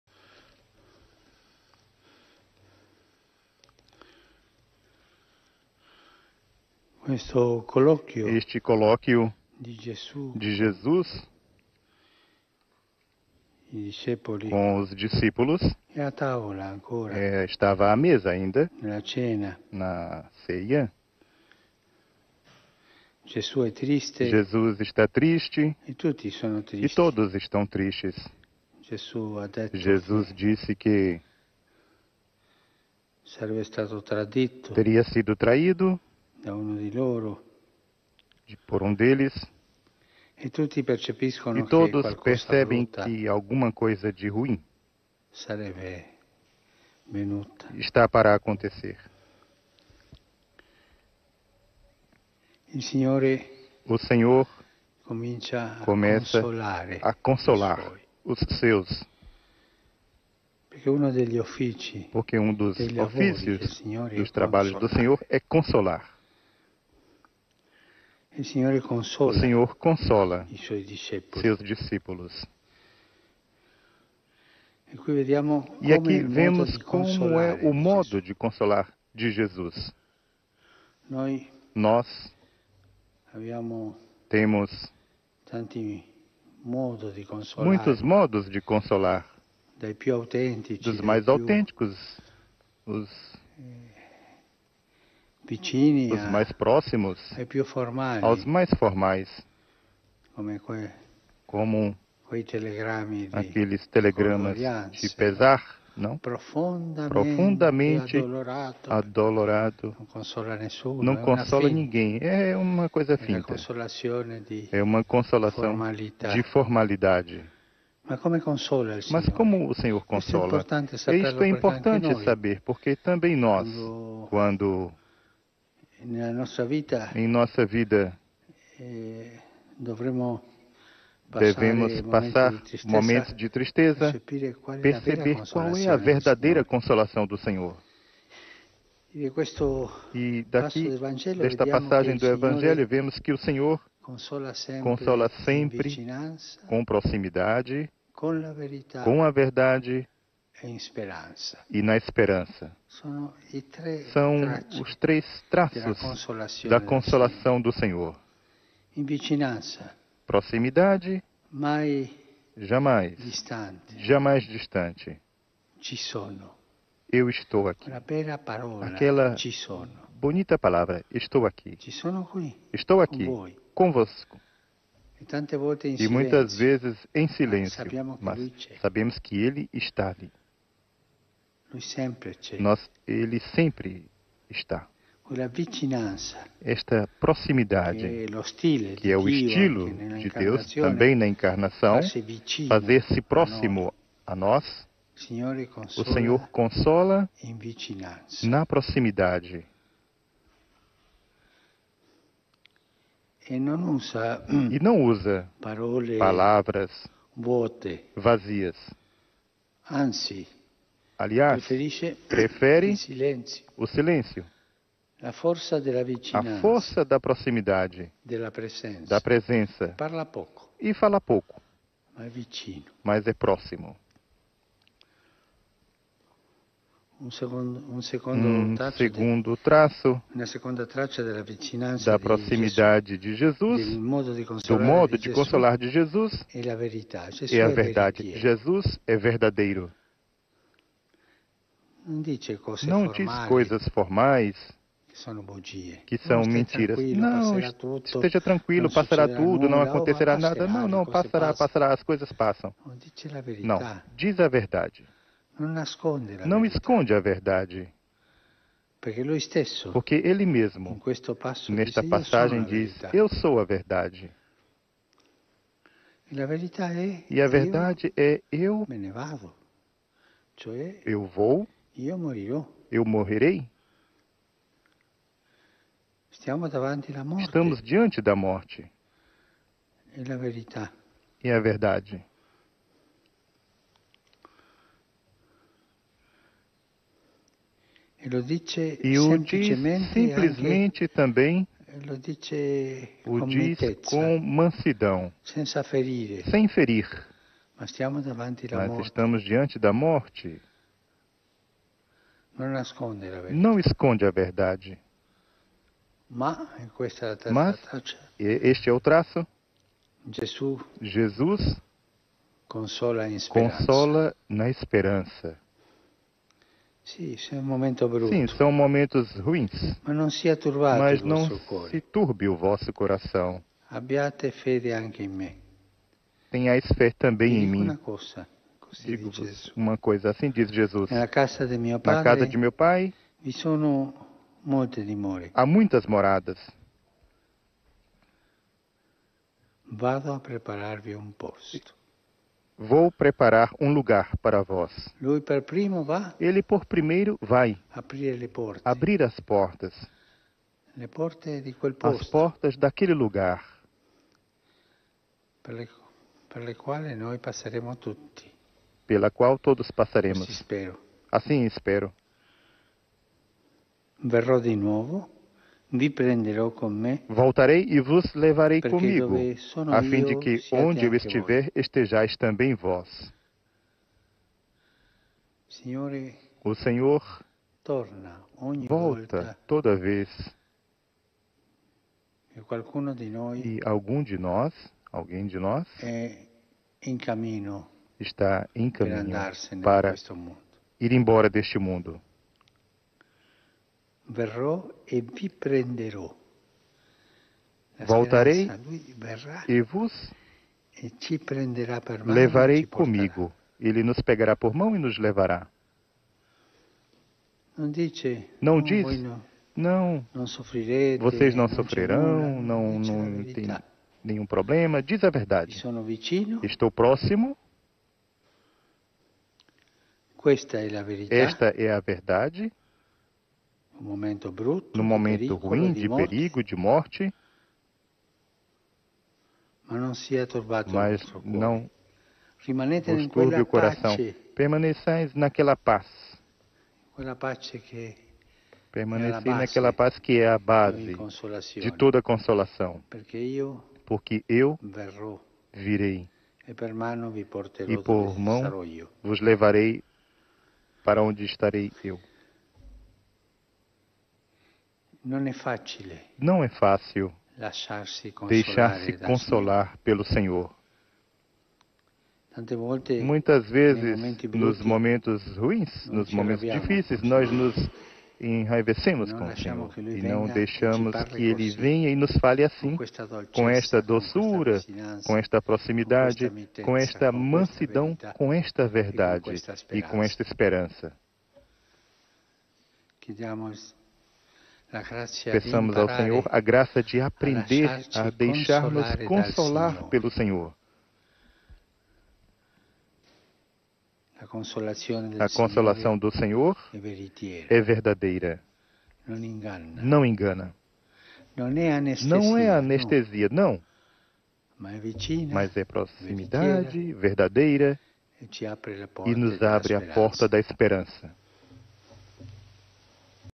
Homilia do Papa Francisco na Missa da Casa Santa Marta - 08/05/2020
Homilia Diária com o Papa Francisco - Capela da Casa Santa Marta - 08 de Maio de 2020

Daily Homily with Pope Francis - Chapel of Casa Santa Marta - 08 May 2020

Homilía diaria con el Papa Francisco - Capilla de la Casa Santa Marta - 08 de mayo de 2020

Omelia quotidiana con papa Francesco - Cappella di Casa Santa Marta - 08 maggio 2020